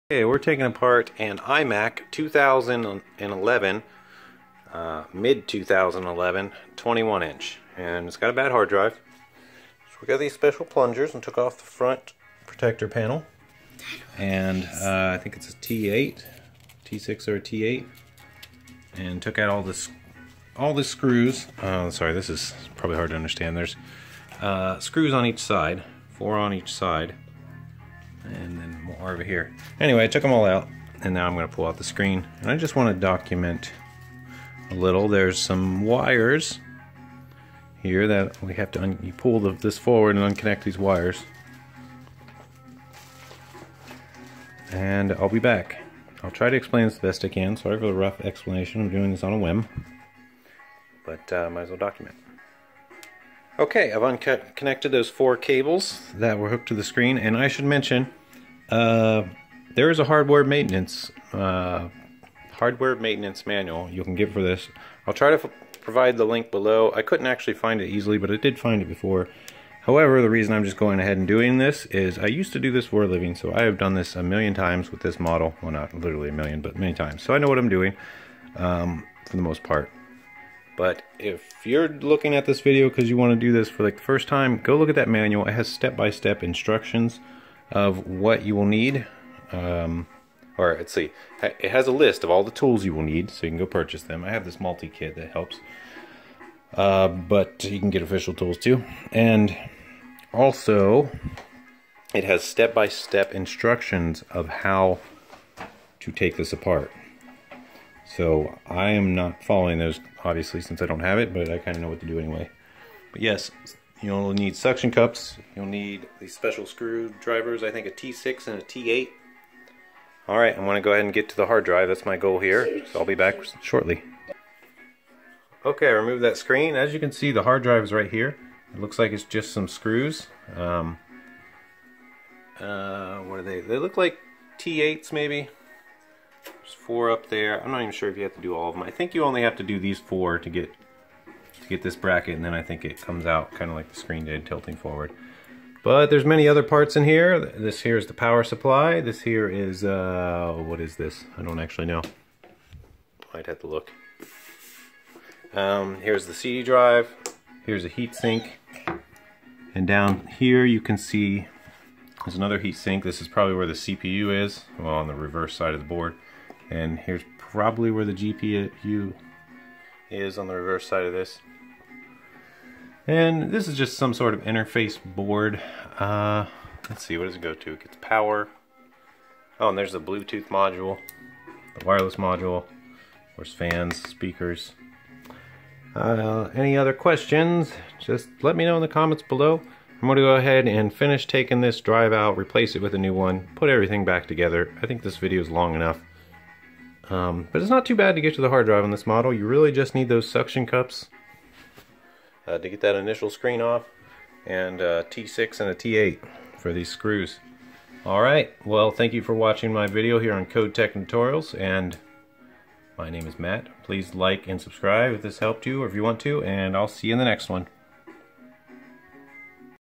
iMac Mid 2011 21.5" Inch - Removing Screen, Changing Hard Drive (HDD) Replacement / Teardown
I was working on this and thought I'd make a quick video in case anyone needs some assistance getting to the hard drive of a 2011 iMac.
You will need plungers and T6, T8, T10 screwdrivers. I skipped over a lot of basics in this video such as you should make sure the iMac is unplugged and has been drained of any power. Make sure you are in a static free area or use a grounding wristbands, etc. All of these basics are also found in the Hardware Maintenance Manual as well if you can manage to find it. I tried again and it didn't come up easily but I will edit this if I find it later or if someone links it to me.